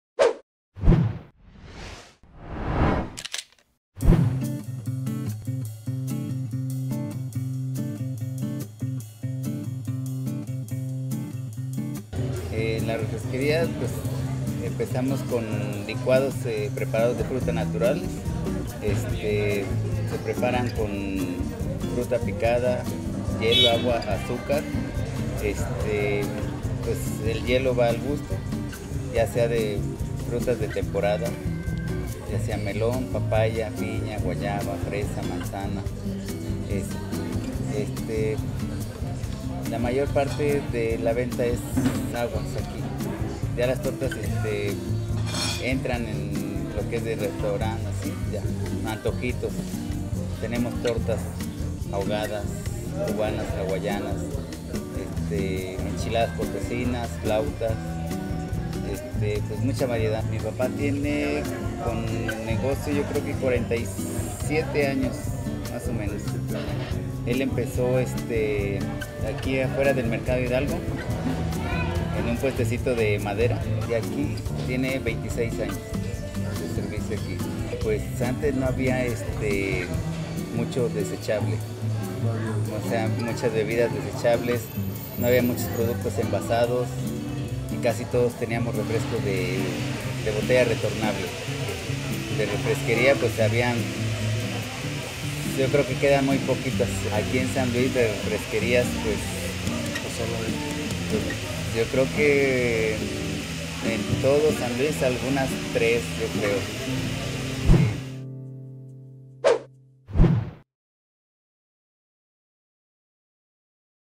Refresquería Acapulco, establecimiento único en SLP
Las “Refresquerías” son un concepto comercial ya prácticamente en extinción en el país.